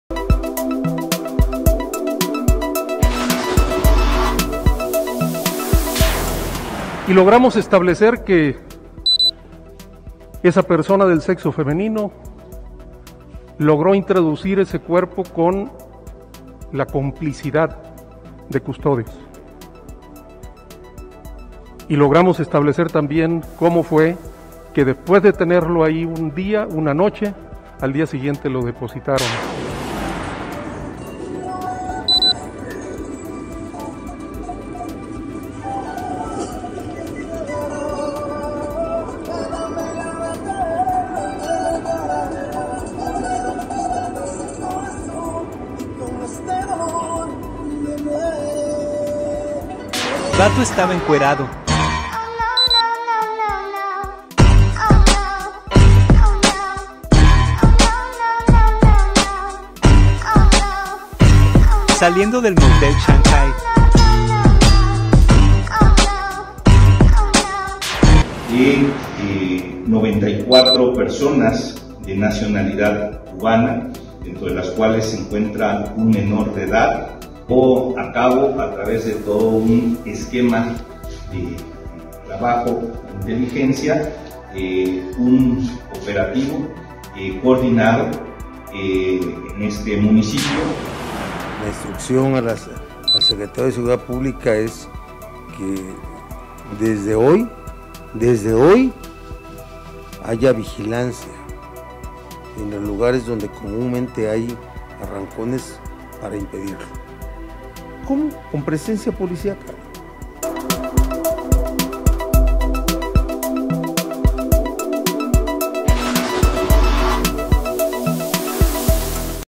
SucesosPuebla | ☀️ Resumen semanal
Bebé Tadeo estuvo un día y una noche en penal de San Miguel 🚨

Desalojan a Pueblos Unidos de planta Bonafont 🏭

El caso del "encuerado del Shanghái" 🏩

En Acajete rescatan a 109 migrantes 🚶🏻

Se frenarán arrancones: Barbosa ❌